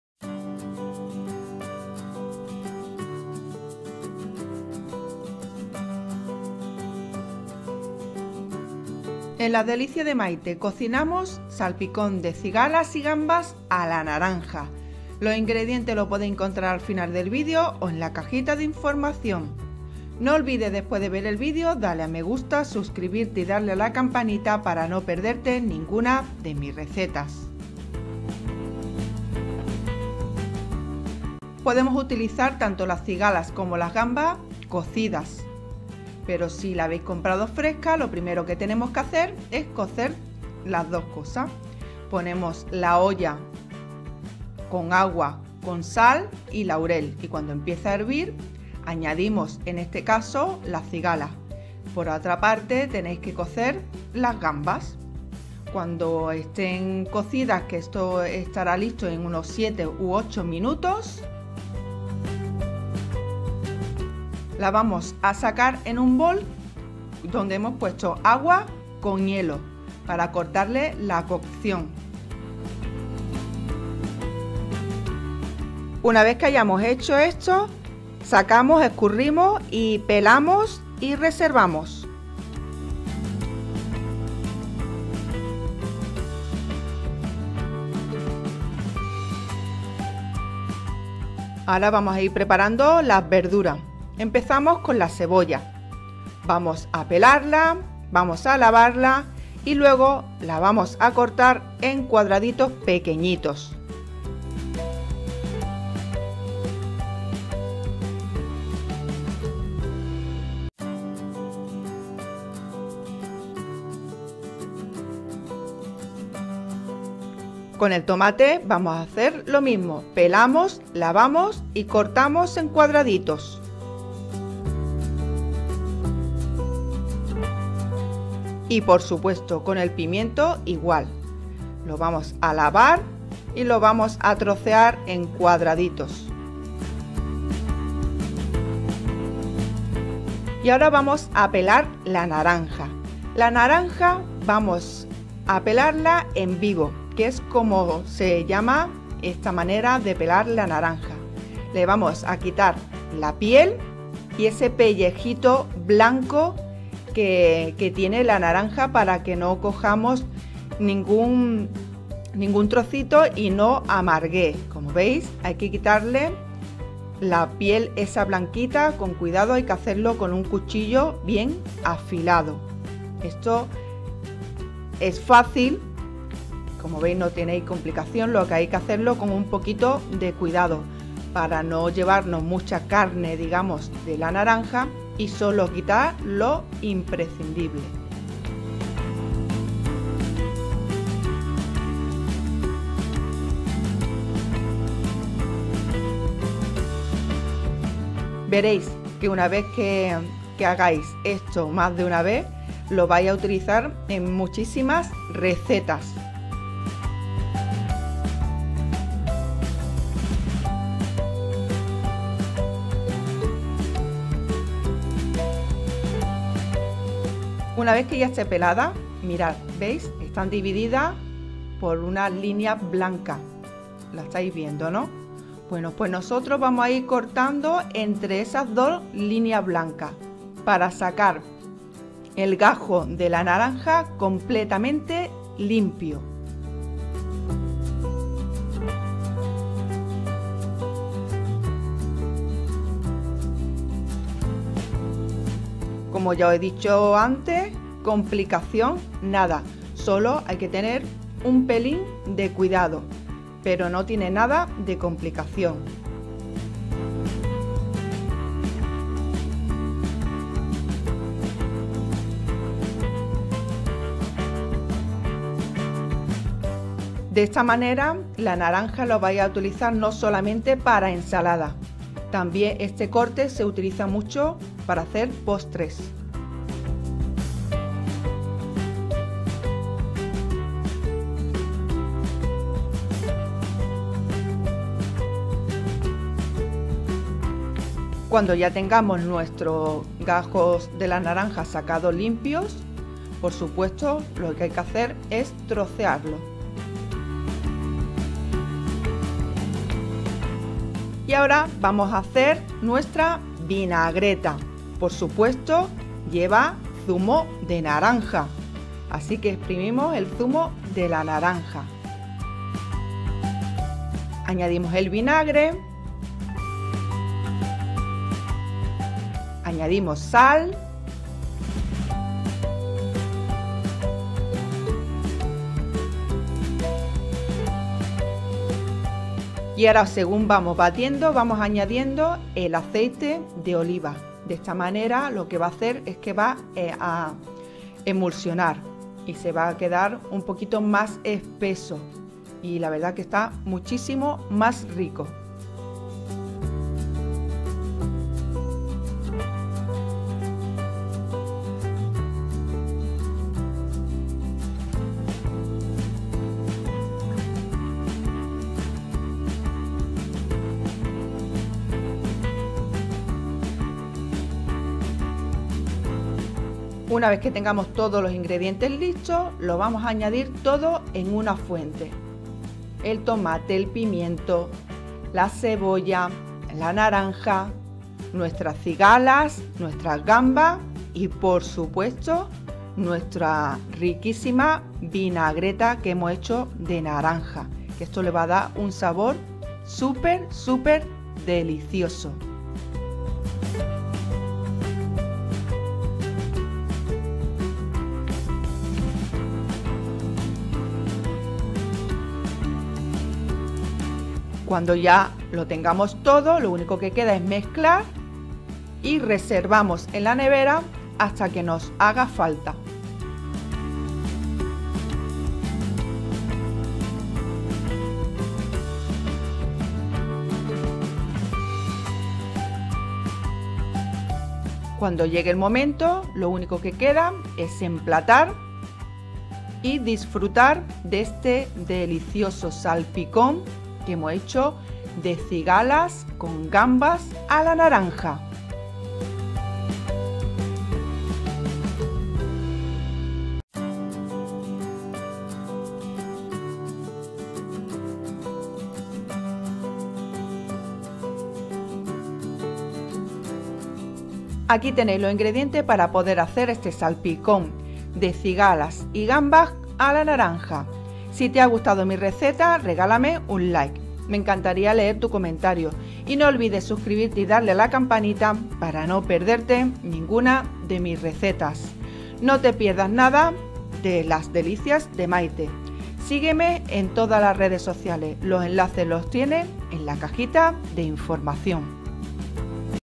SALPICON DE CIGALAS Y GAMBAS A LA NARANJA
Salpicón de cigalas y gambas a la naranja ideal para poner antes del plato principal.

INGREDIENTES:
- Cigalas frescas o cocidas.
- Gambas frescas o cocidas.
- Cebolla.
- Tomates.
- Pimientos verdes.
- Naranjas.
- Zumo de 1 o 2 naranjas.
- Aceite de oliva.
- Vinagre.
- Sal.

INGREDIENTS
- Fresh or cooked lobsters.
- Fresh or cooked prawns.
- Onions.
- Tomatoes.
- Green peppers.
- Oranges.
- Juice of 1 or 2 oranges.
- Olive oil.
- Vinegar.
- Salt.